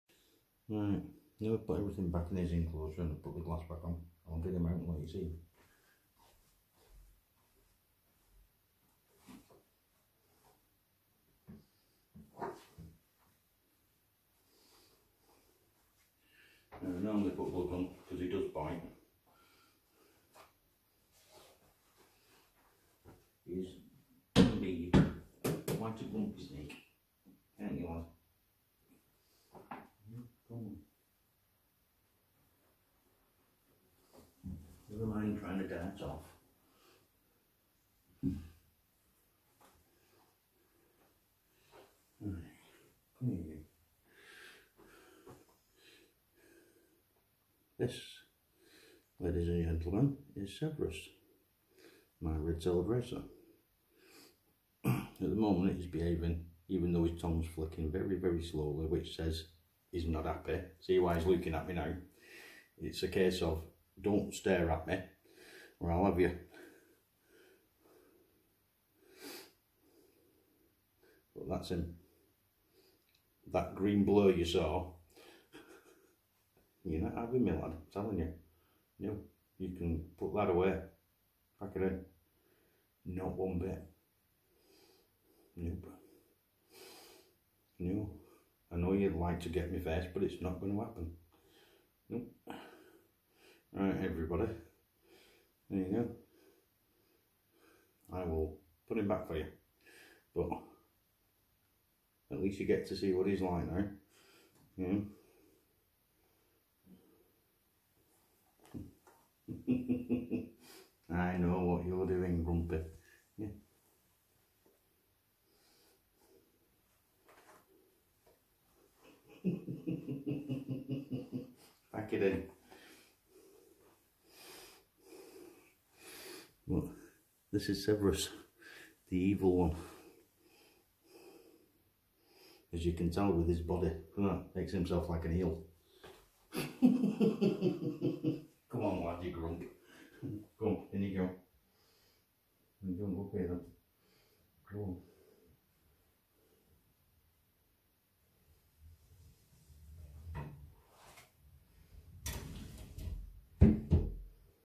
Red tailed racer snake: time for you lovely people to meet severus the evil one: you will see why 🐍
i love this snake but he is so antisocial it does make me laugh,
now these racer snakes are part of the rat snake family so he is naturally just evil so they NOT for the beginner as they are fast moving and quite crafty you need to watch them all the time as you will see by this short video:
thankyou.